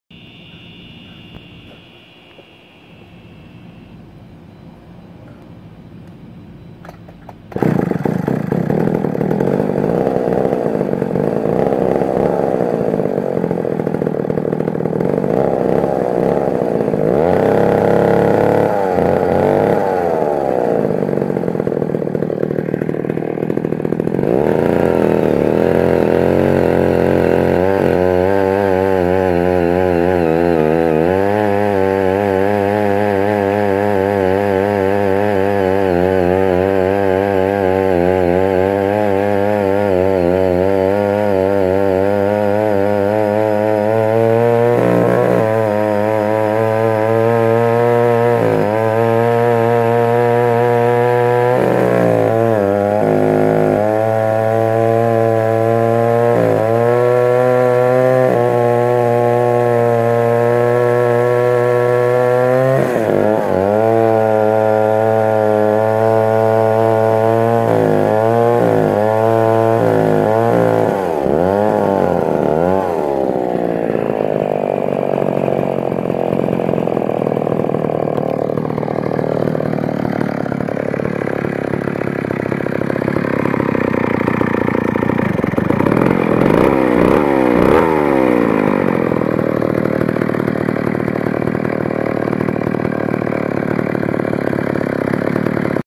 McCulloch 640 cutting 2
Has the eBay stihl 090 carb on it, haven't adjusted the jets and seems to like where it is at. These carbs flanges and impulse holes line up however some of the air boxes are shallower, like this one, and you will need the low clearance tilly carbs fuel line barb section to work on this however saws like the super 44a that have a large air box this carb fits as it is from the box just have to rotate the plastic fuel inlet barb. You may have to make or modify your linkage and change the throttle lever orientation as I had to do and the choke linkage will have to be custom too.